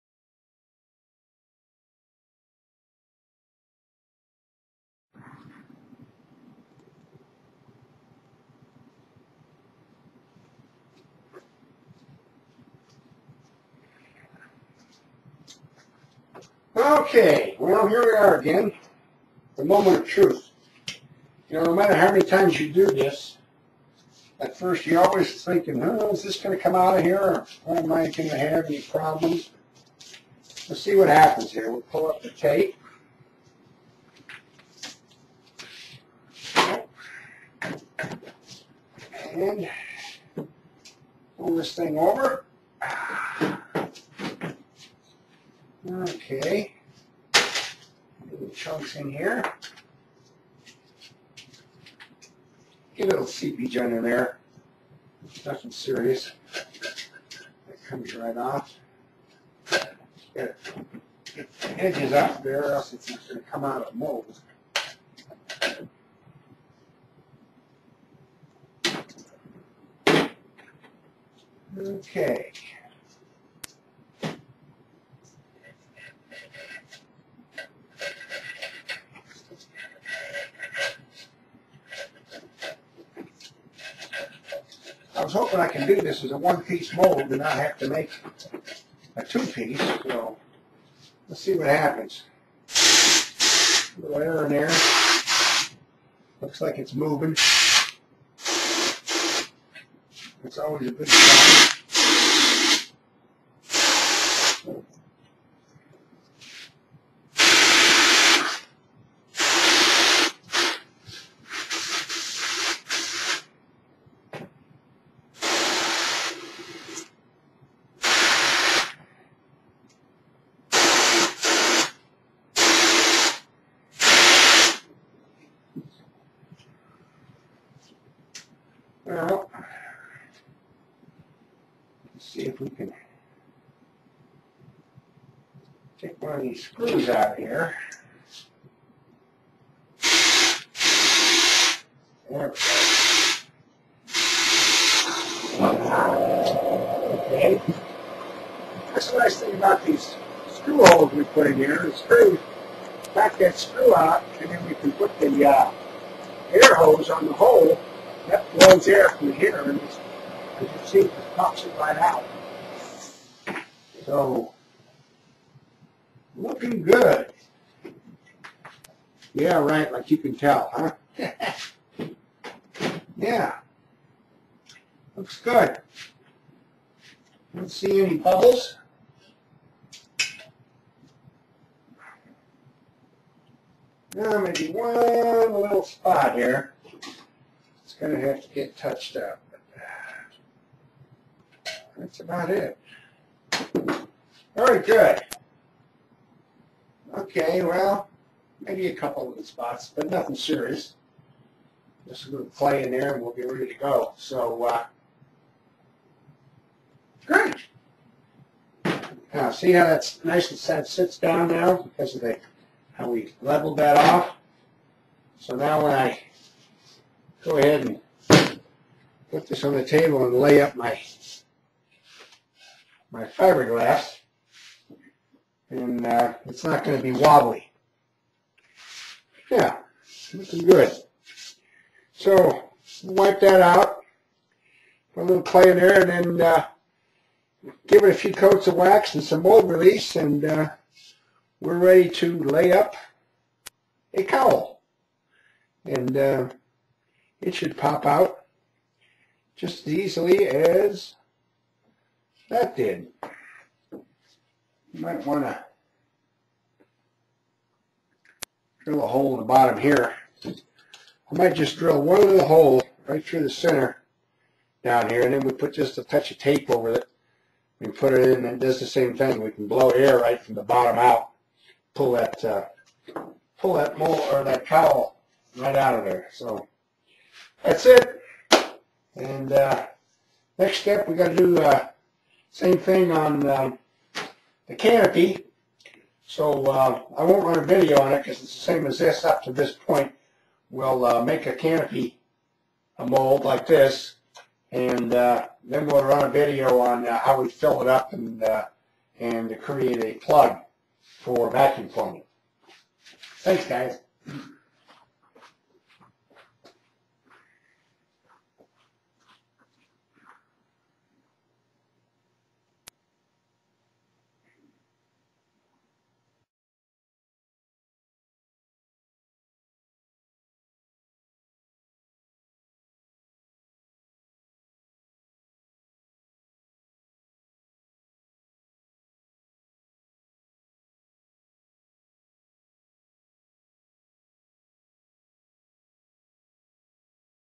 Starship #4 Nacelle Mold Part 4
Casting of nacelle mold for 1/4 scale Beech Starship 2000A--Part 4